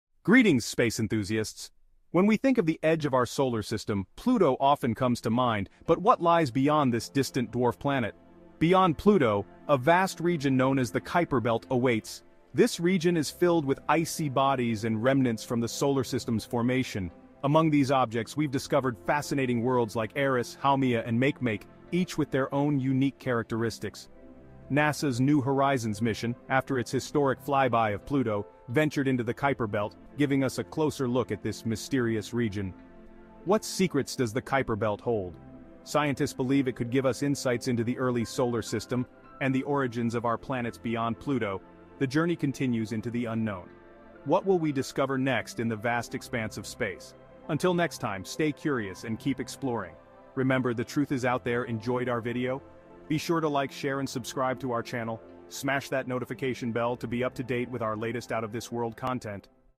Exploring Beyond Pluto: The Mysterious Kuiper Belt
Join us on a journey beyond Pluto as we explore the mysterious Kuiper Belt, home to fascinating icy worlds and remnants from the solar system’s formation. Discover what secrets lie in this distant region and what NASA’s New Horizons mission has unveiled.

Watch our related video about Discovering Exoplanets.